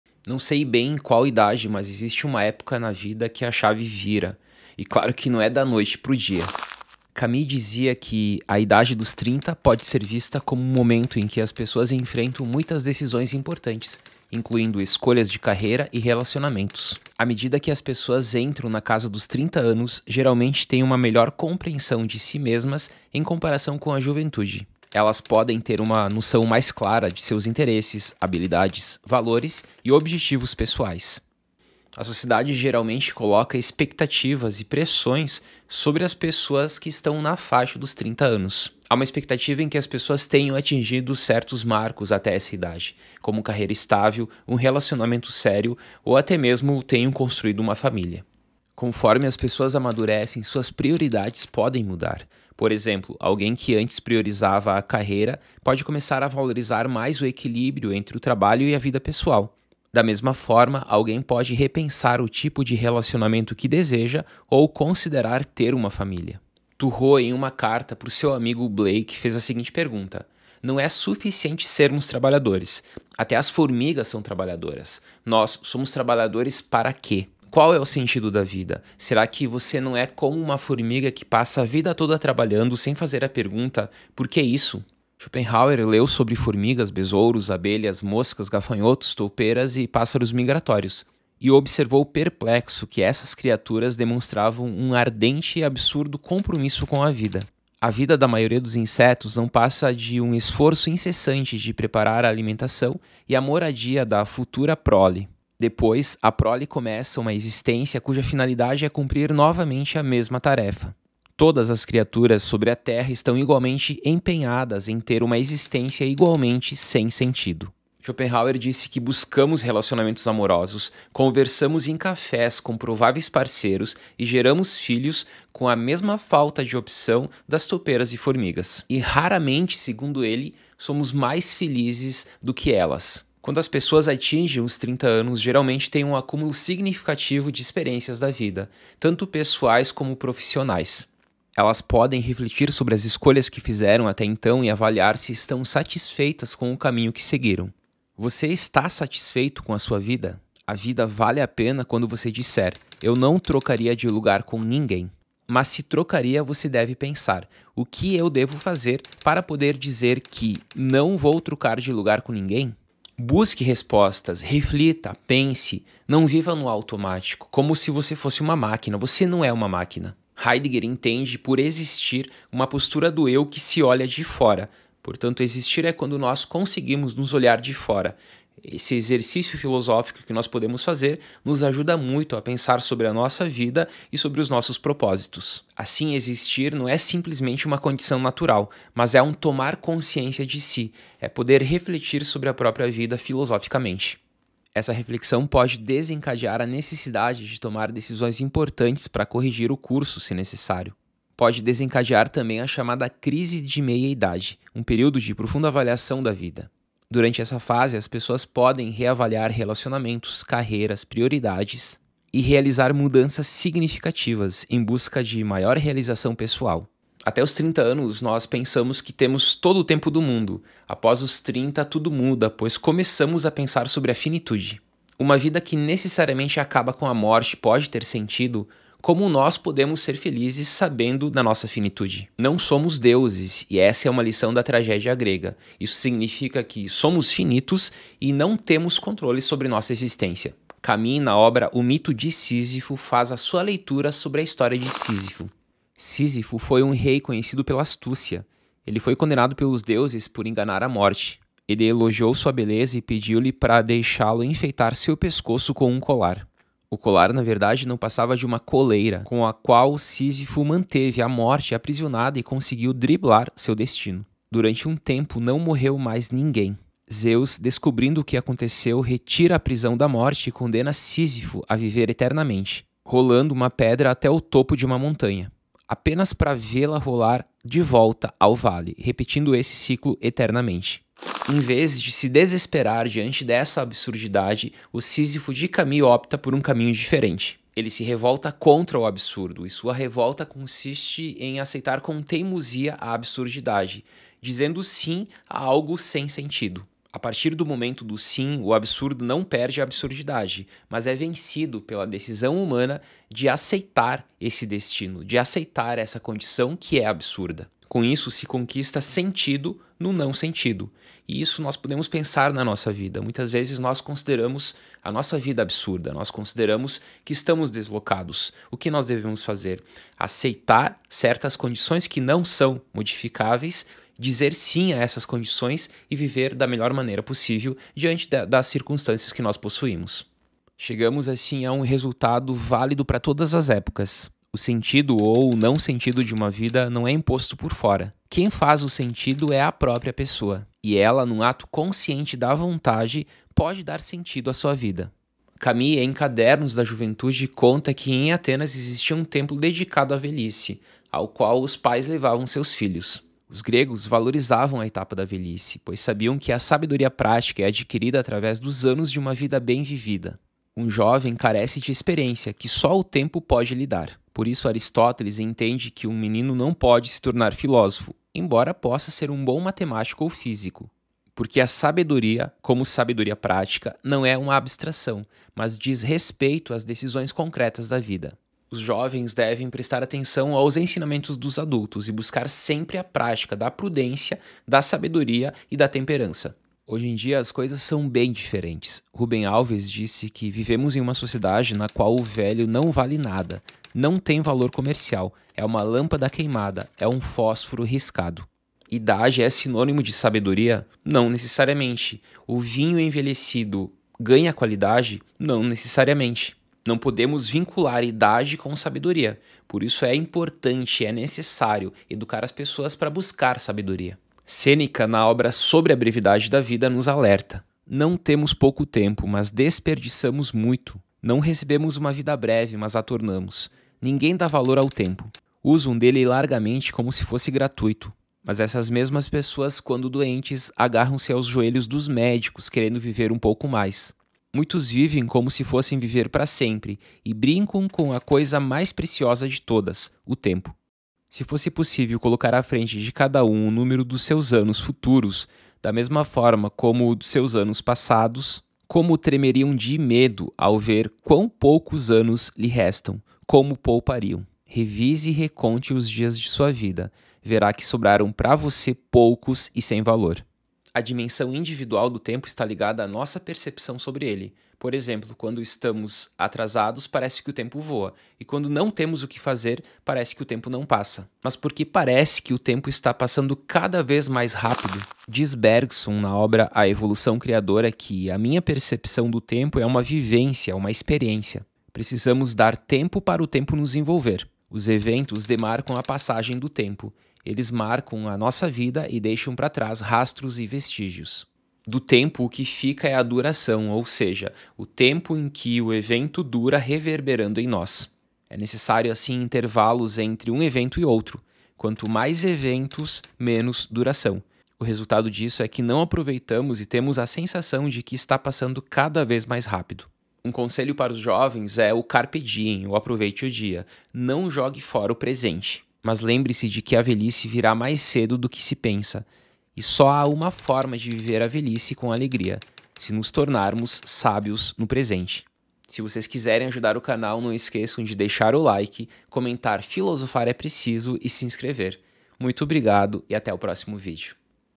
CRISE DA MEIA-IDADE? TUDO MUDA NOS 30 ANOS (Camus)
Neste vídeo, a "fase dos 30 anos" é explorada como um momento crucial de autodescoberta e decisões, enquanto a busca pelo sentido da vida, a sabedoria na velhice e a percepção do tempo são discutidos, culminando na importância de encontrar significado no presente. O mito de Sísifo, de Albert Camus e ideias de filósofos como Henri Bergson, Sêneca e Schopenhauer são usados para ilustrar a complexidade da existência humana e a busca por um propósito autêntico.

00:00 A idade dos 30
01:06 Somos como as formigas?
02:13 Crise de meia-idade e finitude
04:00 O mito de Sísifo
05:45 A velhice e a sabedoria
06:54 Tornamos a vida breve
07:41 A dimensão individual do tempo
08:30 Tornar-se sábio no presente